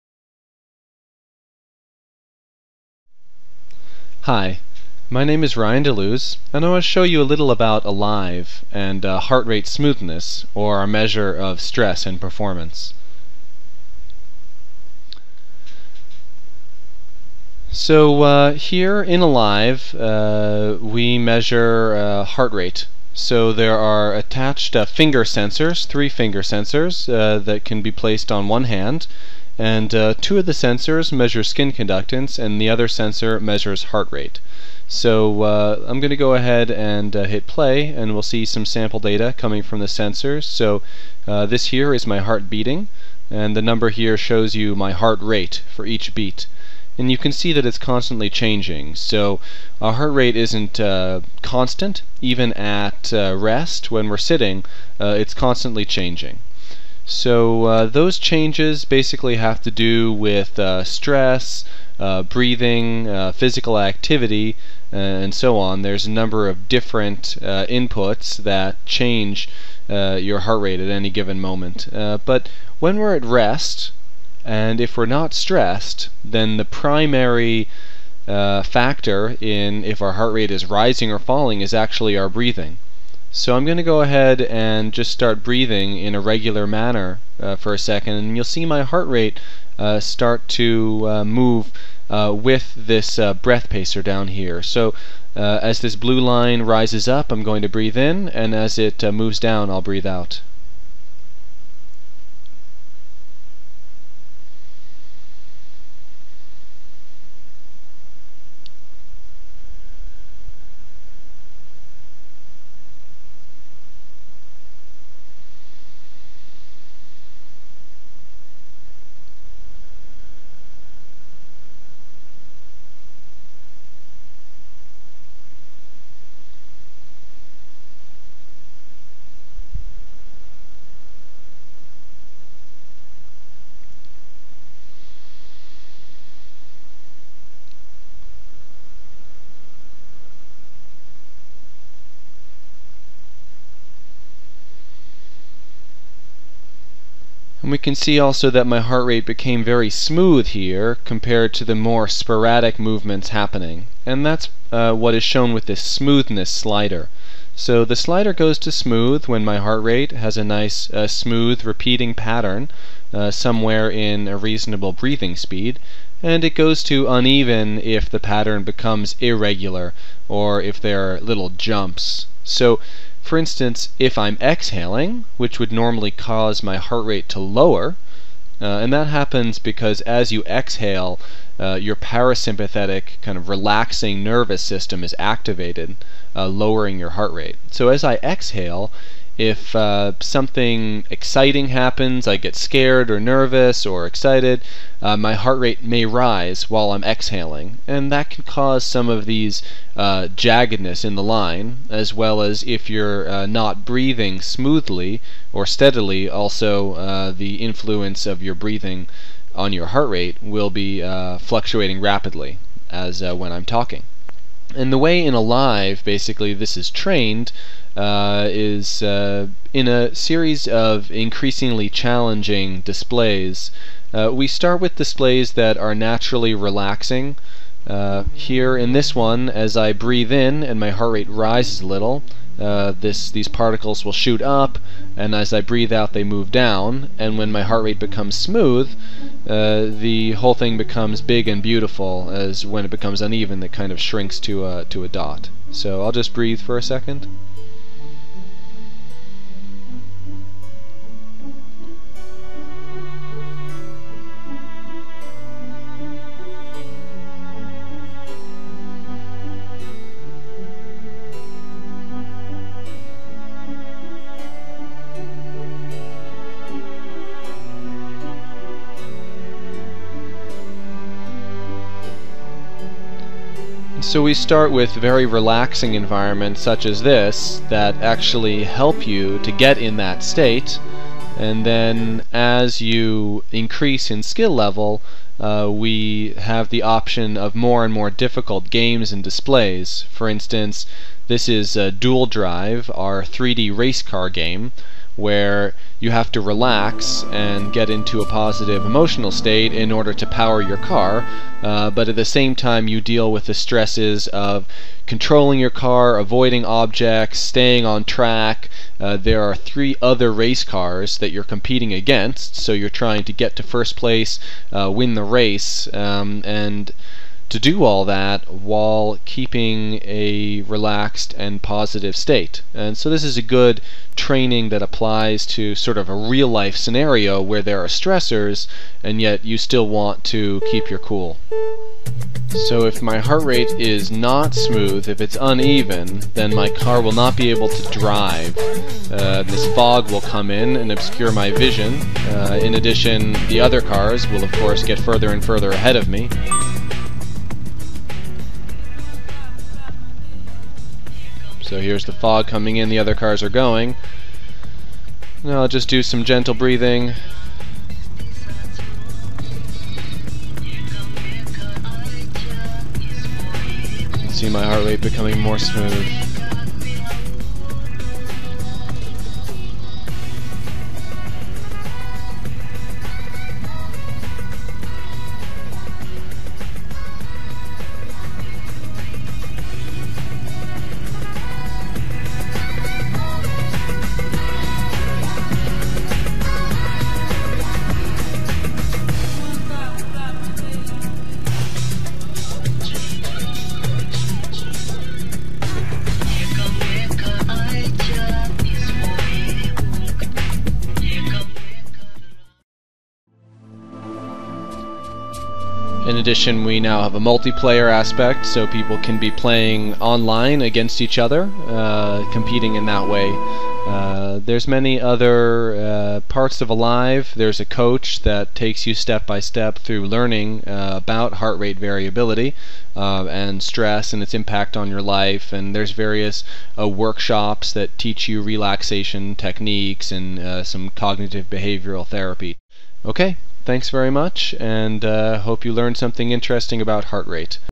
Learn to change your heart rate patterns by breathing
Learn about some of the impacts of stress on heart rate.  Learn how breathing can be used to change heart rate patterns, reduce stress levels and optimize mental and physical performance.